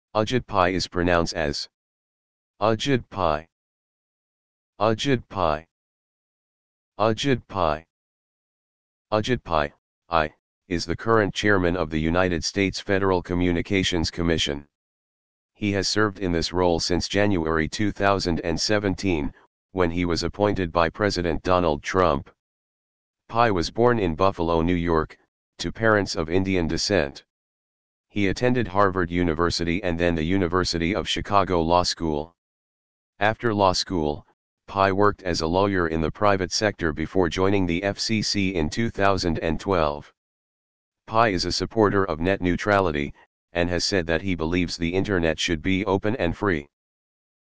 How To Pronounce Ajit Pai
AJIT PAI (eye) is the current Chairman of the United States Federal Communications Commission. He has served in this role since January 2017, when he was appointed by President Donald Trump.

Pai was born in Buffalo, New York, to parents of Indian descent. He attended Harvard University and then the University of Chicago Law School. After law school, Pai worked as a lawyer in the private sector before joining the FCC in 2012.

Pai is a supporter of net neutrality, and has said that he believes the internet should be "open and free."